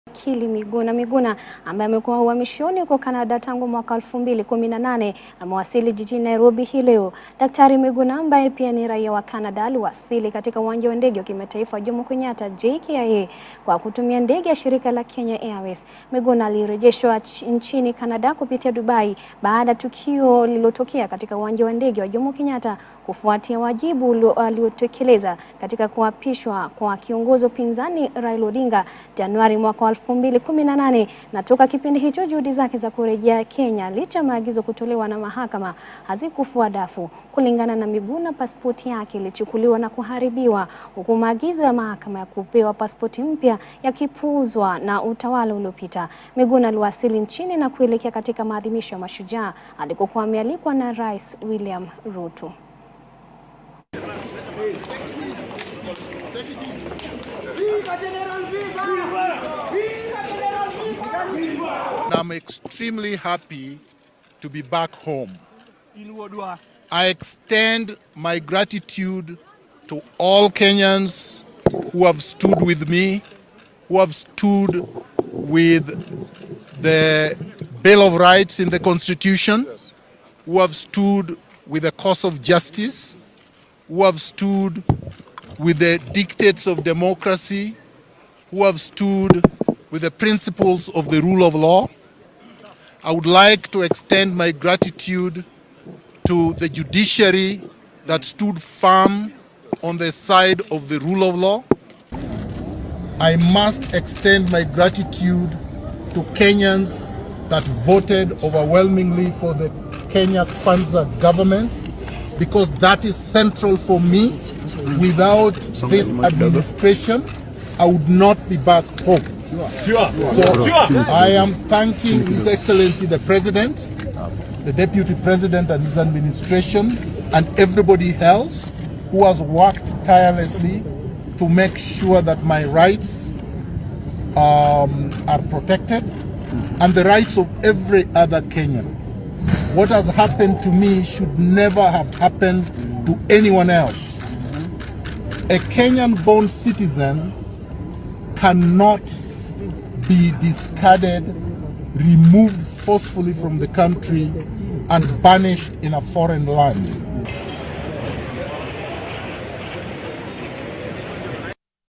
Wakili Miguna awasili nchini kutoka Canada baada ya kufurushwa 2018
Wakili Miguna Miguna ambaye amekuwa uhamishoni huko Canada tangu mwaka wa alfu mbili kumi na nane, amewasili jijini Nairobi hii leo.